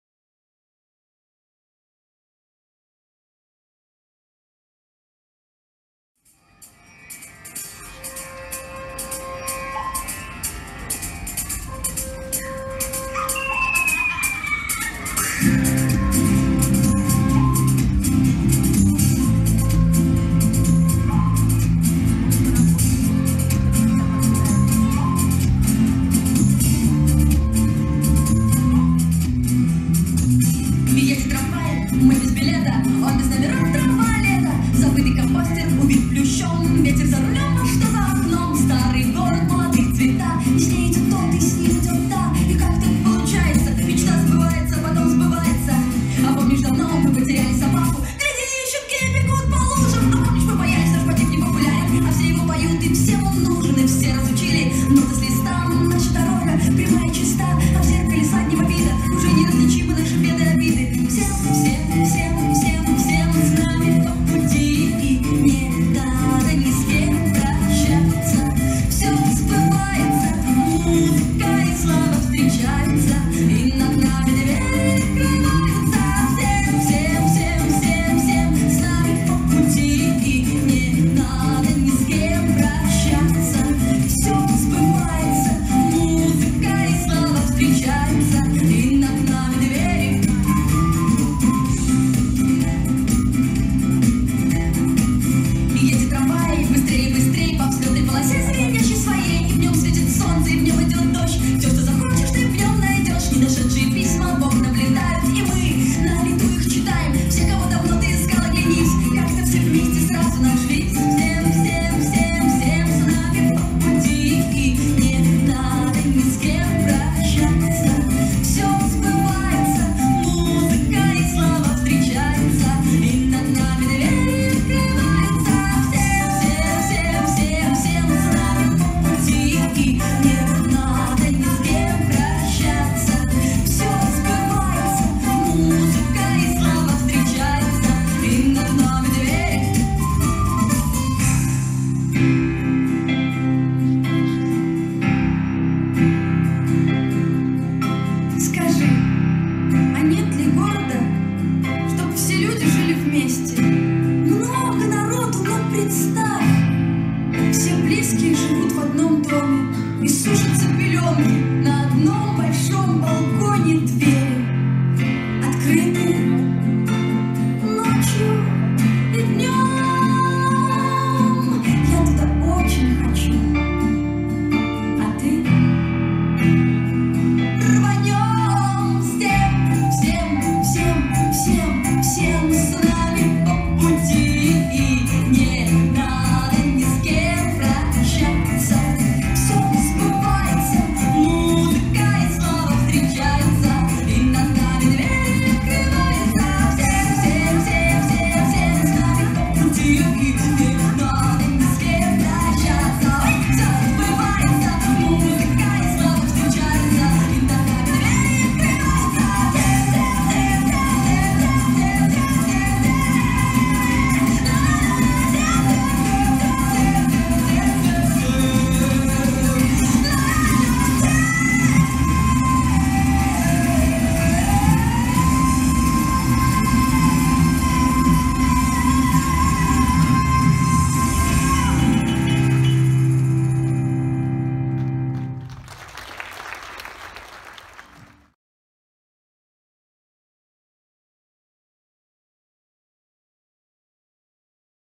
ХХореографический ансамбль "Эксперимент". Волшебный трамвай | РАДУГА ДЕТСТВА
Хореографический ансамбль "Эксперимент". Волшебный трамвай/ Хакасия, ЦДТ г. Абакана.